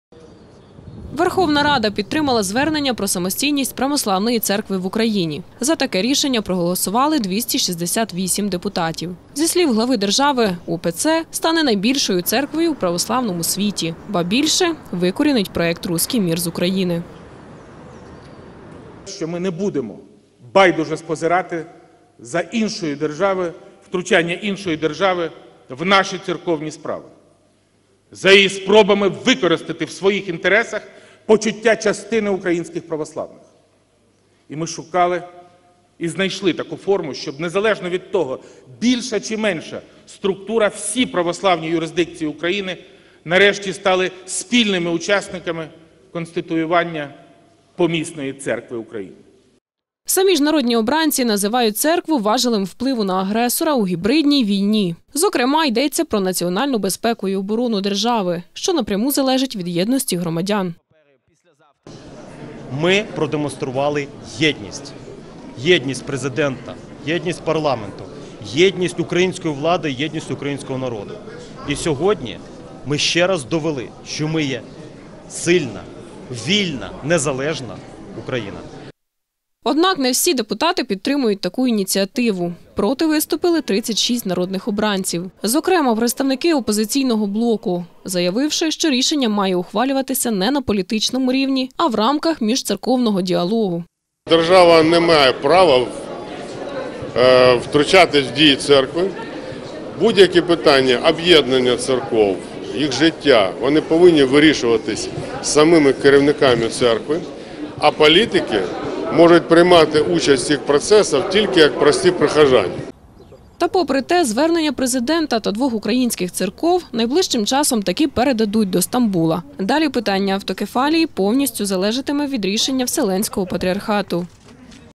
Рада поддержала обращение о самостоятельности православной церкви в Украине
Украинский парламент поддержал обращение Порошенко о самостоятельности православной церкви в Украине. За данное решение свои голоса отдали 268 народных депутатов. По словам Порошенко, украинская православная церковь станет крупнейшей церковью в православном мире и искоренит влияние России на украинских верующих.